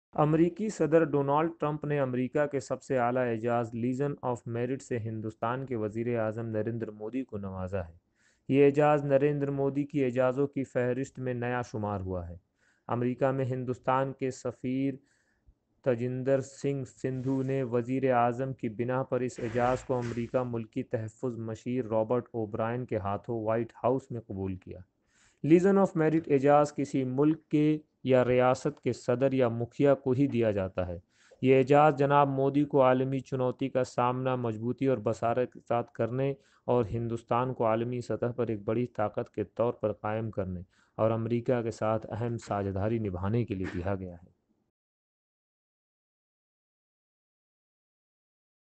US president Trump presents Legion of Merit to PM Modi
امریکی صدر ڈونلڈ ٹرمپ نے امریکہ کے سب سے اعلیٰ اعجاز لیزر آف میرٹ سے ہندوستان کے وزیر اعظم نریندر مودی کو نوازا ہے یہ اعجاز نریندر مودی کے کی اجاز کی فہرست میں نیا شمار ہوا ہے، امریکہ میں ہندوستان کے سفیر ترنجیت سنگھ سندھو نے وزیراعظم کی بنا پر اس اعجاز کو امریکی ملکی تحفّظ مشیر روبرٹ اوبرائن کے ہاتھوں وائٹ ہاؤس میں قبول کیا،
 لیزر آف میرٹ اعجاز کسی ملک کے یہ ریاست کے صدر یا مکھیاں کو ہی دیا جاتا ہے، یہ اعجاز جانب مودی کو عالمی چونوتی کا سامنا مجبوت اور بصارت کے ساتھ کرنے اور ہندوستان کو عالمی سطح پر ایک بڑی طاقت کے طور کو قایم کرنے، اور امریکہ کے ساتھ اہم سجھیدری نبھانے کے لیے دیا گیا ہے

US President Donald Trump has awarded the highest miracle of the United States, the Legion of Merit, to Indian Prime Minister Narendra Modi. The White House acknowledged the miracle at the hands of US National Security Adviser Robert O'Brien.
 The laser of merit miracle is given only to the president or bee of this state of a country. It has been given to play an important role with the United States